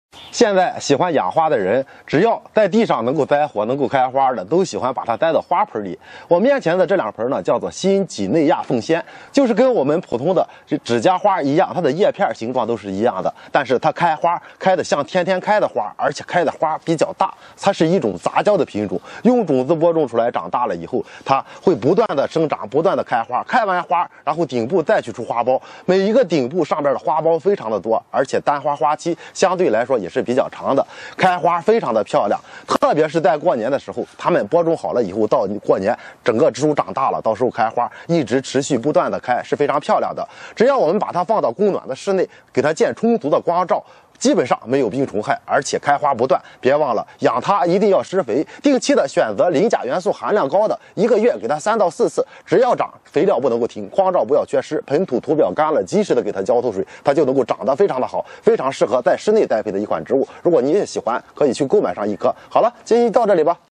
【養養養花花花】顏色多彩的新幾內亞鳳仙，冬天這樣養，節日也會開出美麗花朵
花友們常見的傳統鳳仙花，一般是作為一年生的草本植物來栽培的，比較喜歡光照，通常在夏秋季節開花，新幾內亞鳳仙原產於新幾內亞島上的高山區，雖然也是鳳仙花科的植物，但是是多年生的草本植物，夏季怕暴晒，秋冬氣溫合適的話，也會很好地開花。